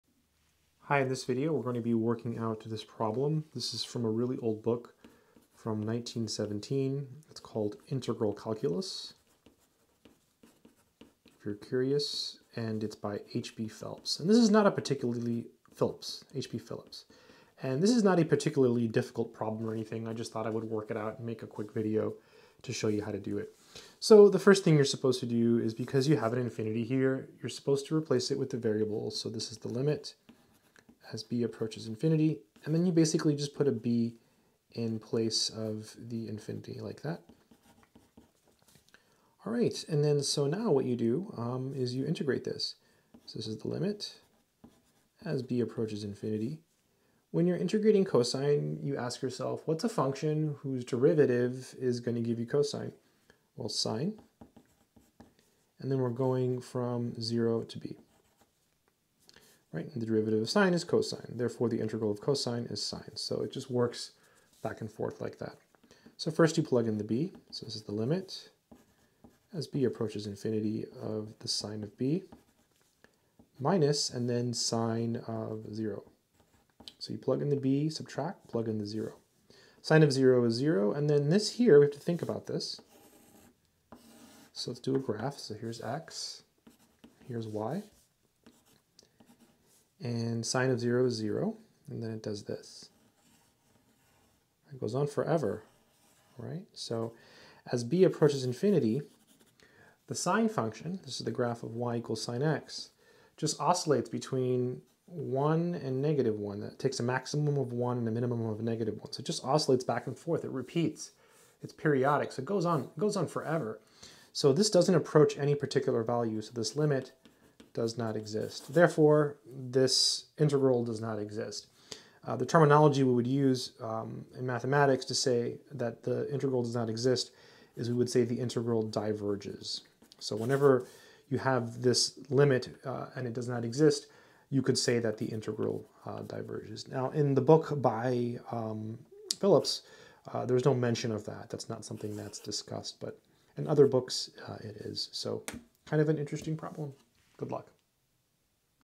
Integral of cos(x) from 0 to infinity
In this problem I work out the integral of cos(x) from 0 to infinity. This is a problem from a very old book called Integral Calculus. It was written by H.B. Phillips and it was published in 1917.

The book on amazon
The pencils I used in this video
The paper I used in this video